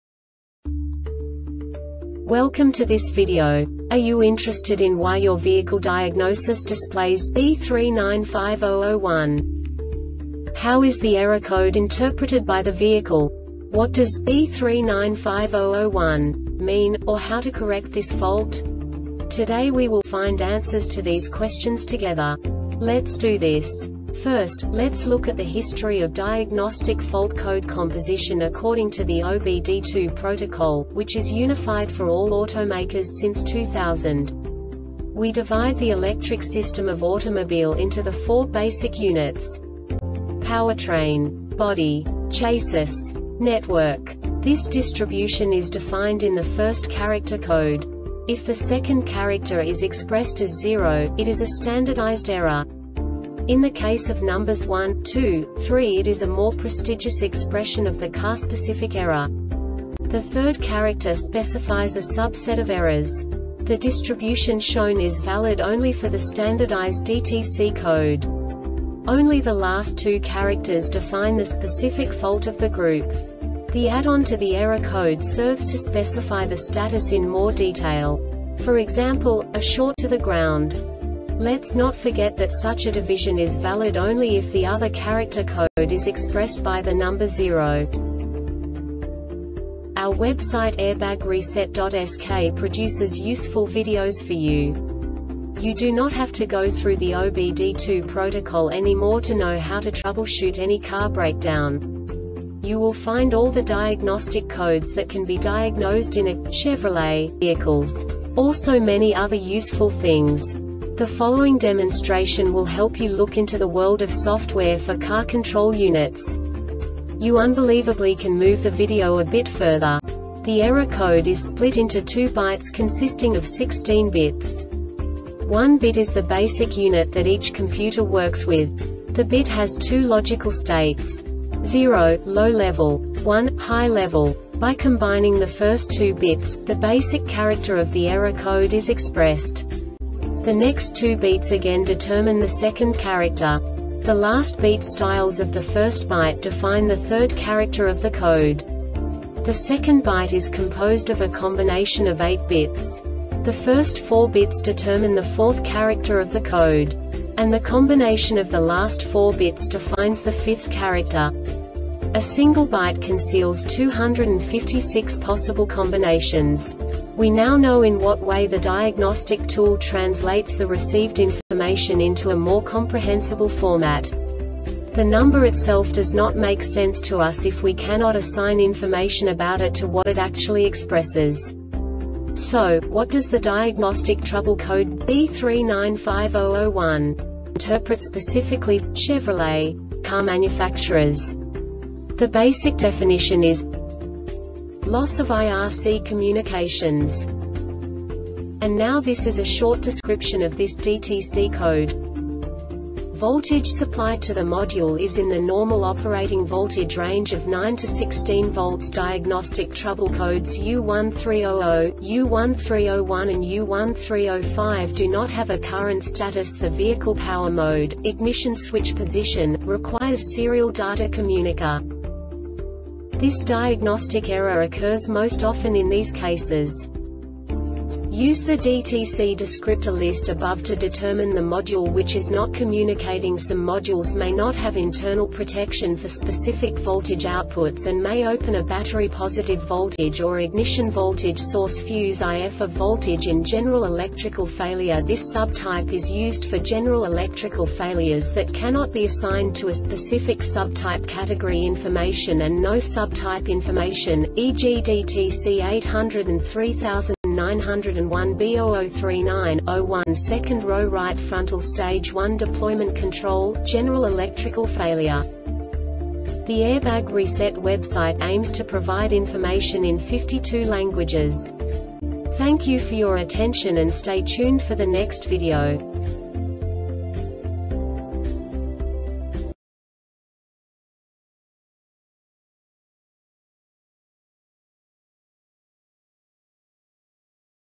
DTC Chevrolet B3950-01 Short Explanation
Contents:
0:21 Basic DTC analysis according to OBD2 protocol standard.
1:48 Insight into programming
2:58 A diagnostically understandable description of the Diagnostic Trouble Code
3:05 Basic error definition

Make:
Chevrolet
Code:
B3950 01
Definition:
Loss of IRC Communications
Description: